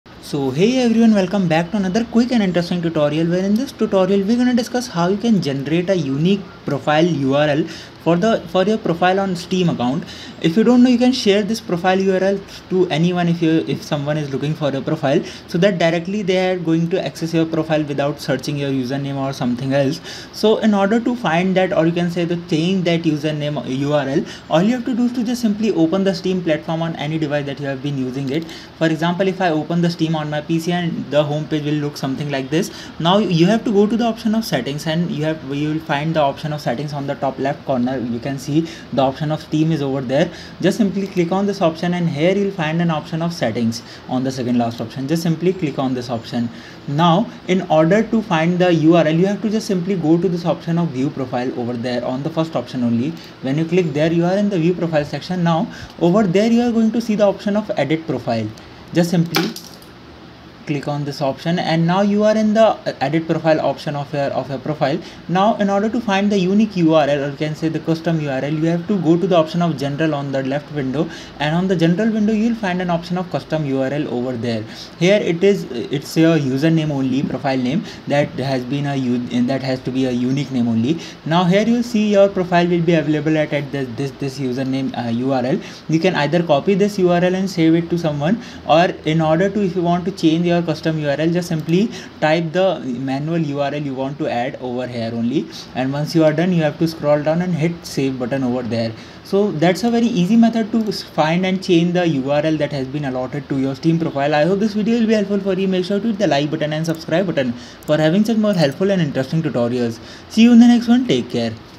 How to Generate a Sharable Link or URL for Steam Profile | How to Get Profile Link on Steam [EASY]
Want to share your Steam profile with friends or showcase your gaming achievements? Generating a sharable link for your Steam profile is the way to go! In this simple tutorial, we'll show you how to create a shareable URL for your Steam profile, making it easy to connect with others and show off your gaming prowess.

Join us as we walk you through the steps to generate a sharable link for your Steam profile.

In this tutorial, we'll guide you on how to access your Steam profile settings and navigate to the profile URL section. Learn how to generate a unique URL that you can share on social media, gaming communities, or with friends through messaging platforms.

Discover the benefits of having a sharable link, such as making it convenient for others to find and add you on Steam without having to search for your username.

Please note that creating a sharable link for your Steam profile is a fantastic way to connect with fellow gamers and expand your gaming network.

Watch the full tutorial to learn how to generate a sharable link for your Steam profile and share your gaming adventures with ease.